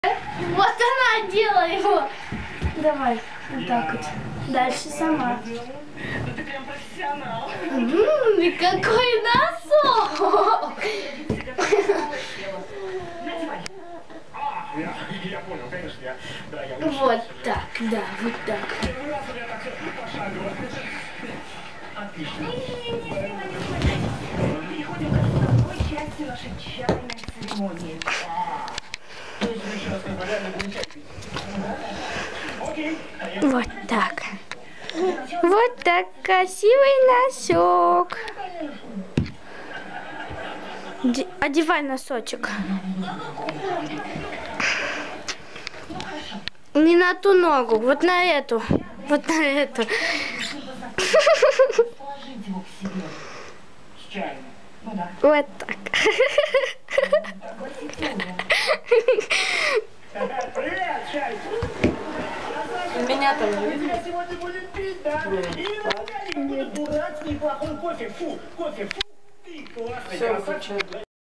Как правильно одевать носки , yjcrb
Как правильно одевать носки .  Дочка учится одевать носочки , а няня над ней прикалывается !